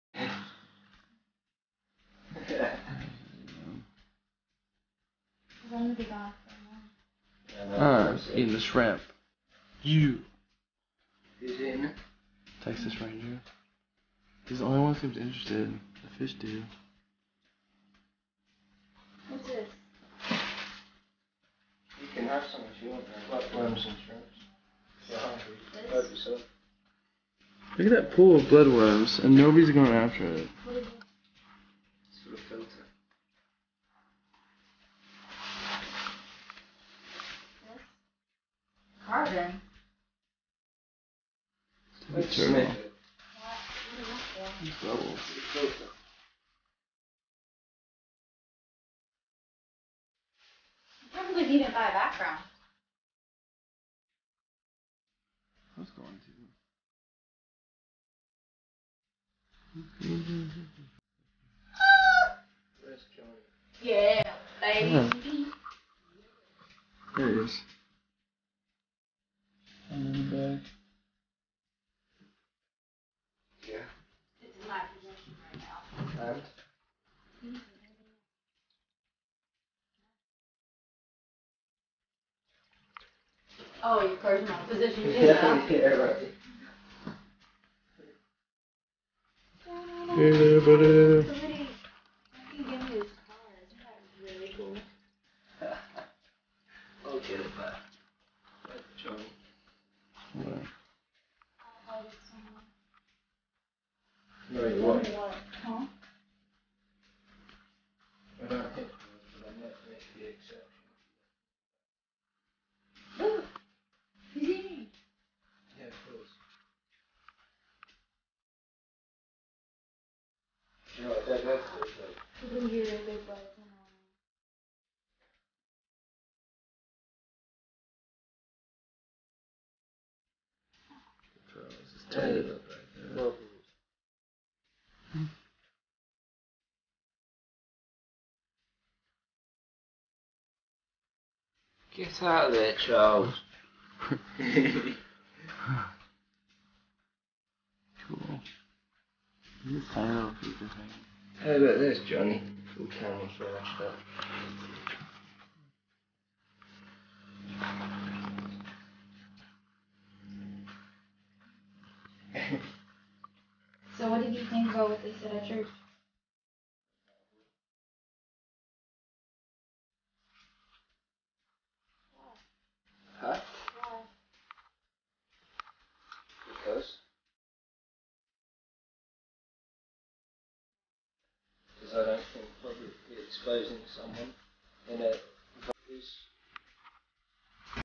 Turtle Tank Revamped! With Texas Ranger and Johnny the Piranha
New rocks and All kinda new plants! Also My new Piranha Johnny! And my New Cumberland Turtle Texas Ranger